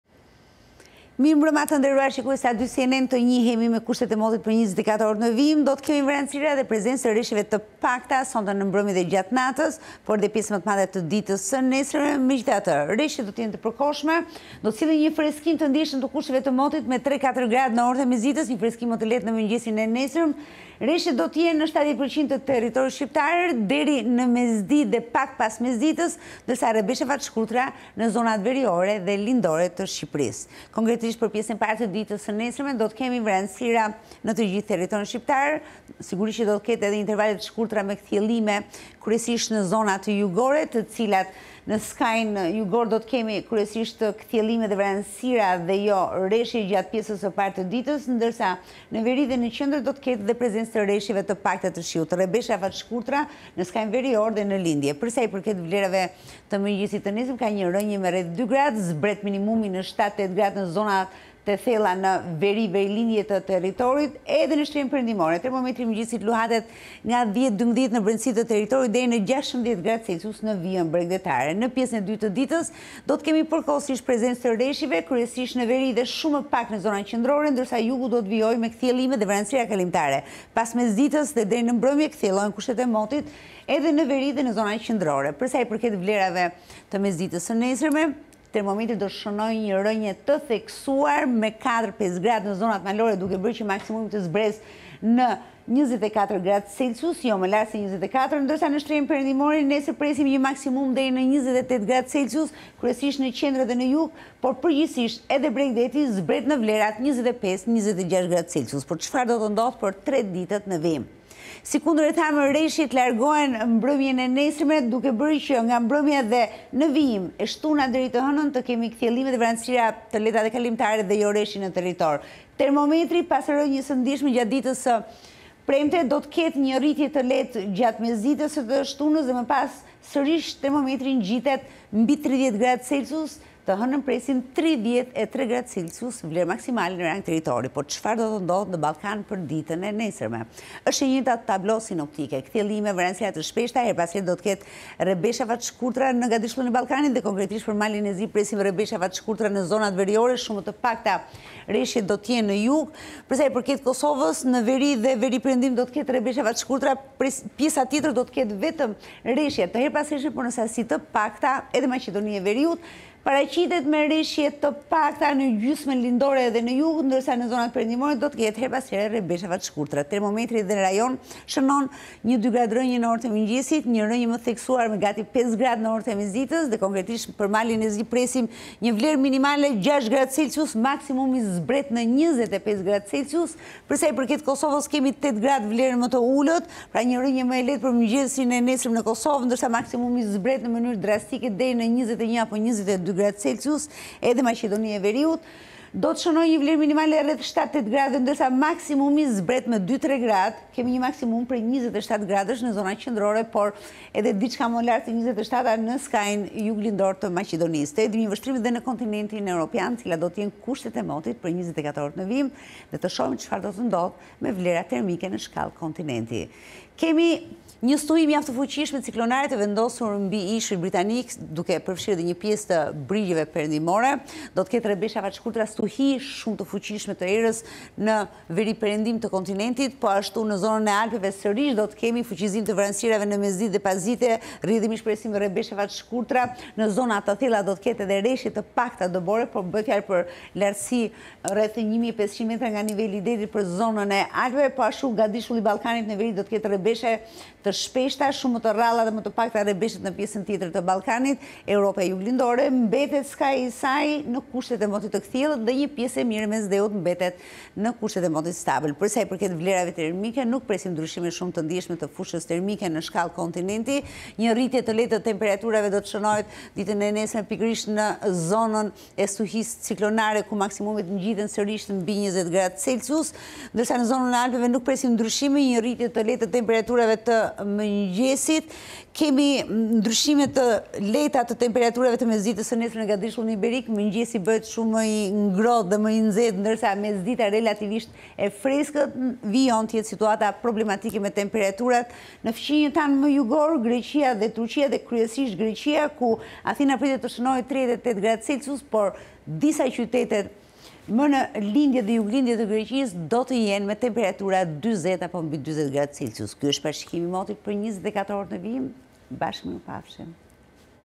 Shiu freskon motin me 3-4 °C - Parashikimi i motit, 13/06/2024
Parashikimi i motit nga meteorologia e A2 CNN, Tanja Porja.